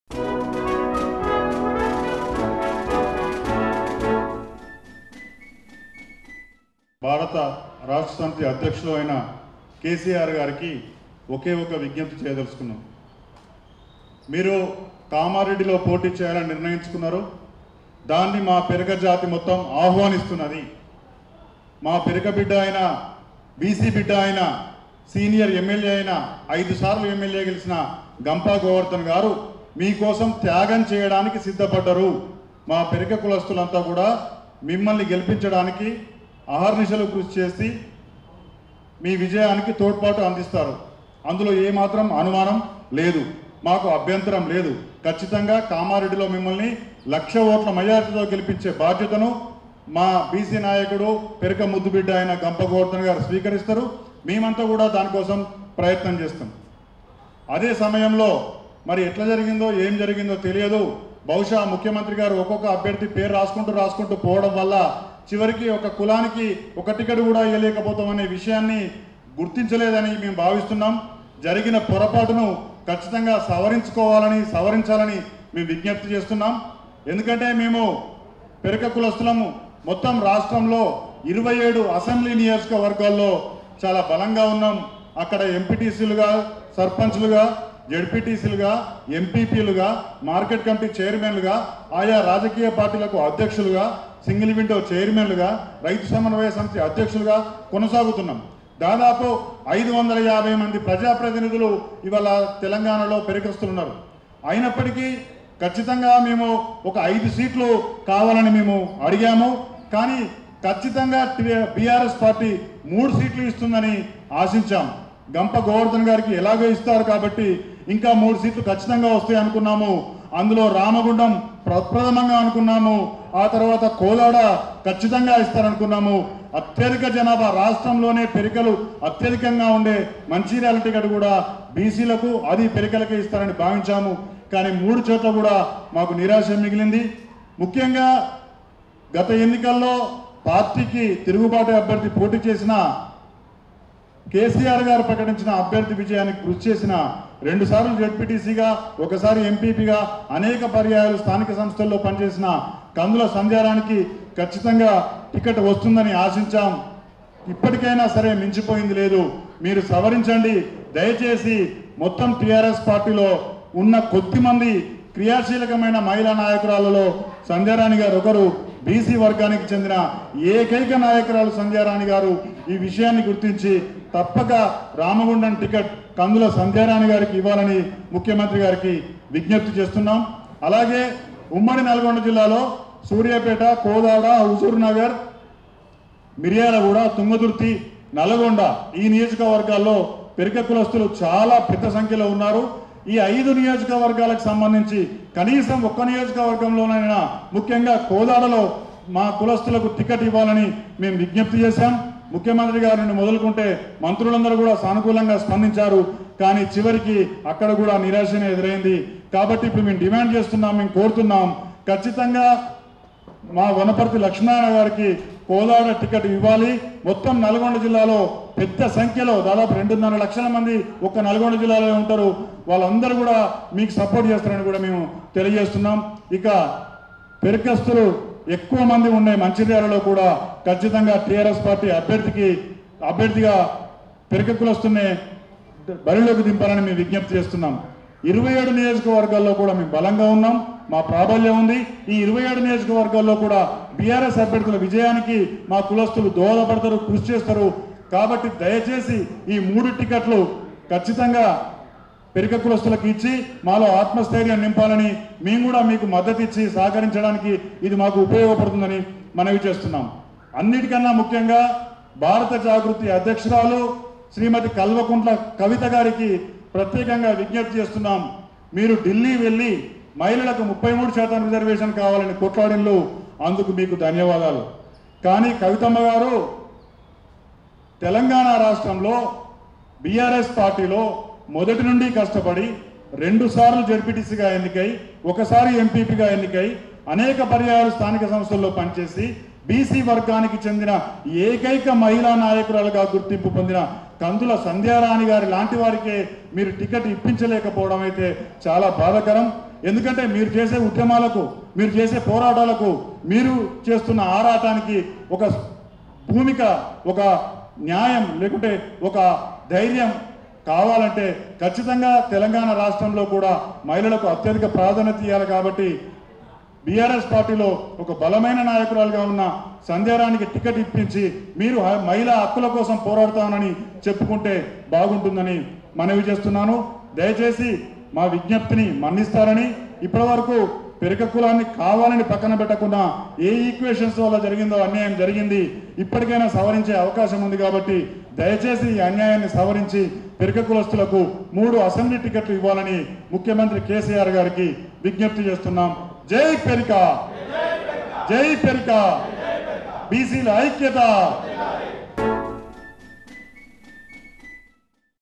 1లక్ష మెజారిటీ తో కేసీఆర్ ను గెలిపిస్తాం | CM KCR Win 1 lakh Majority In KamaReddy | Telugu Popular
1లక్ష మెజారిటీ తో కేసీఆర్ ను గెలిపిస్తాం |  CM KCR Win 1 lakh Majority In KamaReddy | Telugu Popular

కామారెడ్డి లో

     మా పెరిక జాతి మద్దతు తో
        1లక్ష మెజారిటీ తో
       కేసీఆర్ ను  గెలిపిస్తాం